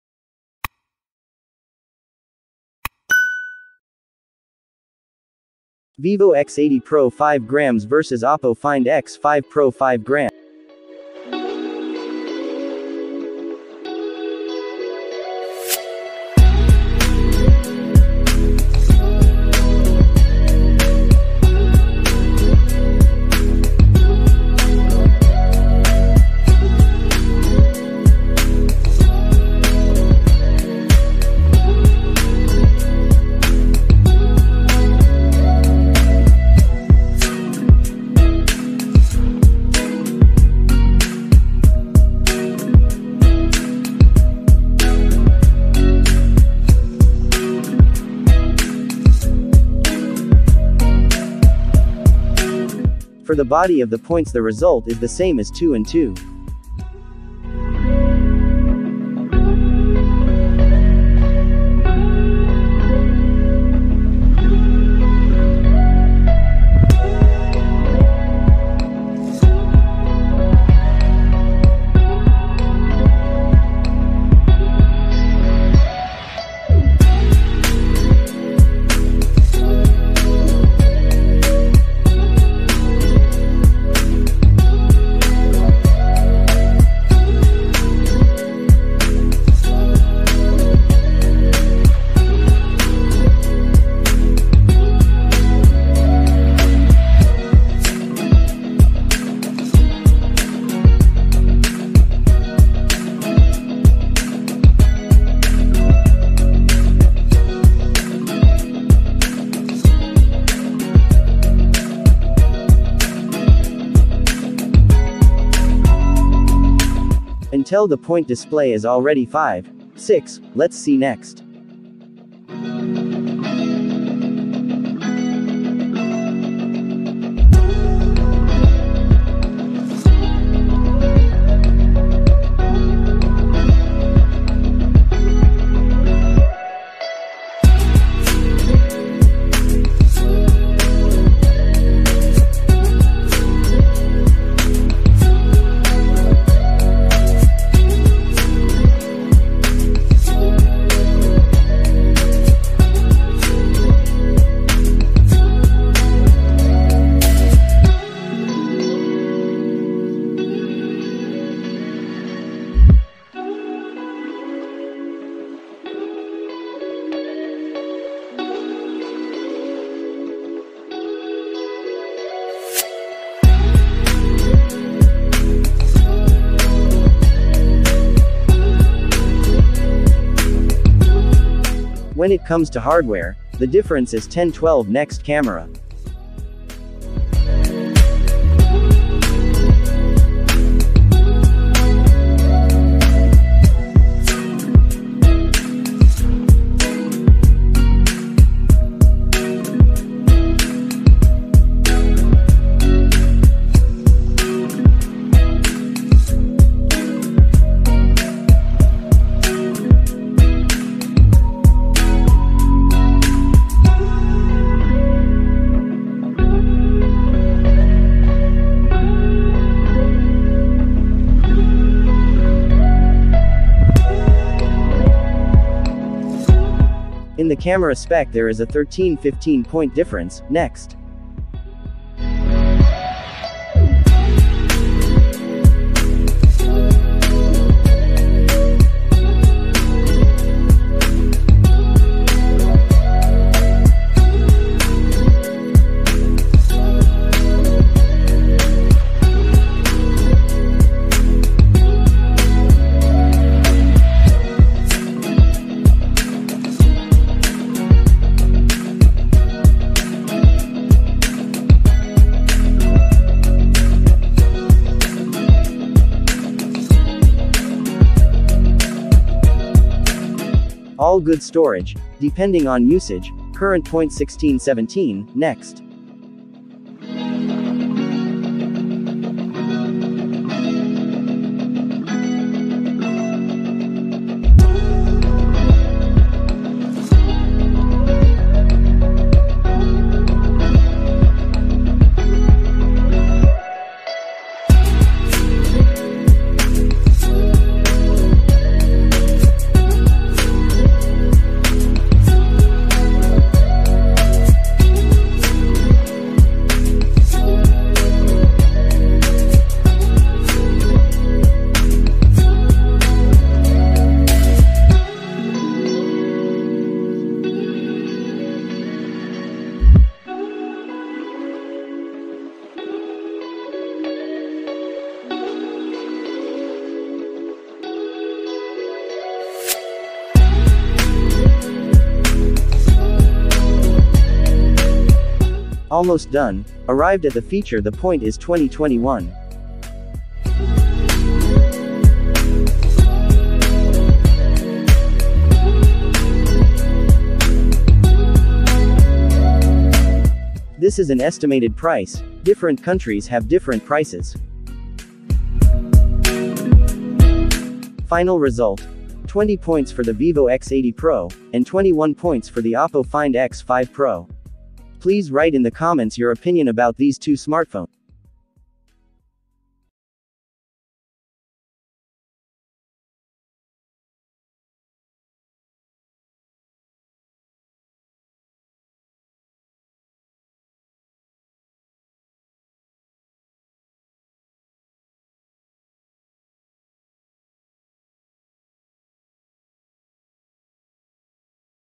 Vivo X80 Pro 5G vs Oppo Find X5 Pro 5G | COMPARE
Full comparison of Vivo X80 Pro 5G vs Oppo Find X5 Pro 5G
including:
body, height, width, thickness, weight, display type, display refresh rate, ppi density, sreen resolution, display size, aspect ratio, water resistant, screen protection, hardware, processor (CPU), chipset, gaming (GPU), operating system, battery, fast charging, camera, back camera, video recording, selfie photos, selfie videos, storage, memory card, ufs, ram, internal storage, usb port, headphone jack, radio, nfc, fingerprint, bluetooth, and price estimates.